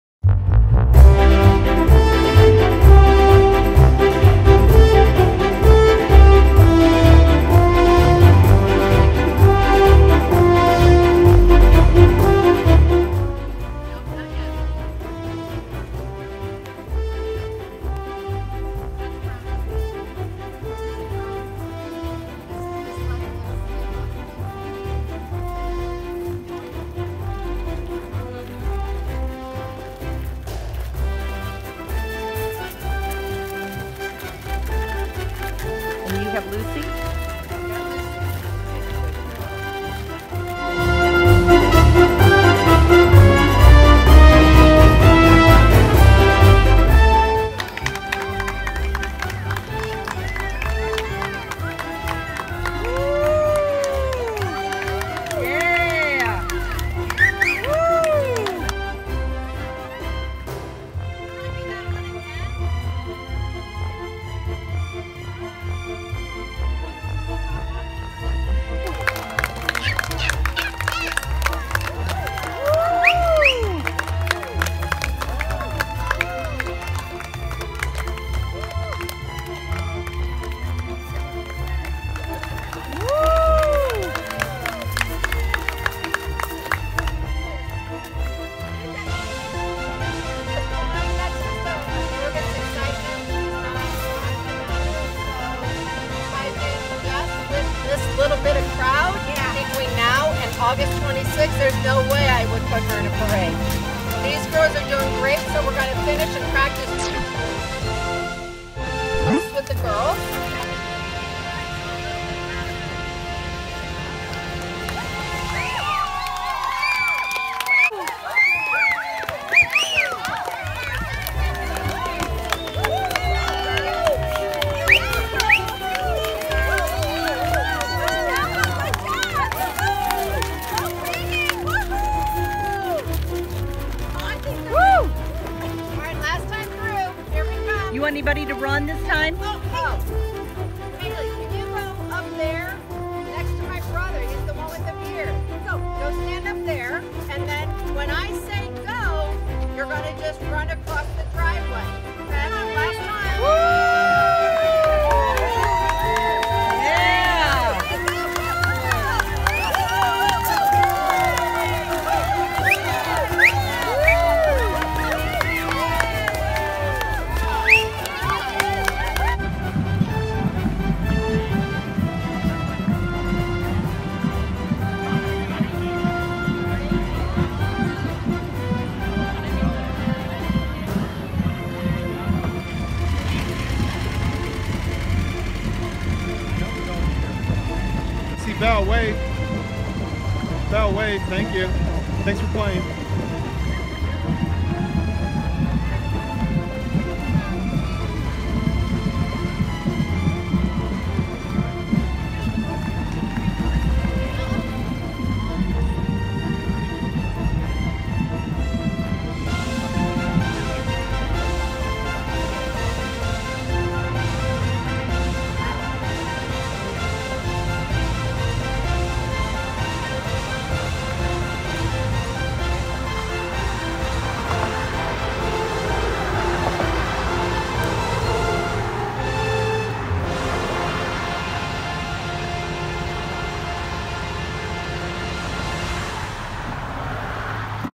So you think your horse can be in a parade?
Parades are fun for kids of all ages. Horses, not so much. They’re loud, balloons are waving, marching bands blaring and lots of noisy clapping.

No matter how well behaved your horse may be, it may not be cut out for walking in a parade.

This video shows how we practiced with our horses before accepting an invitation to be in a parade.

We put them through the paces multiple times before we felt confident they were up to the challenge🥳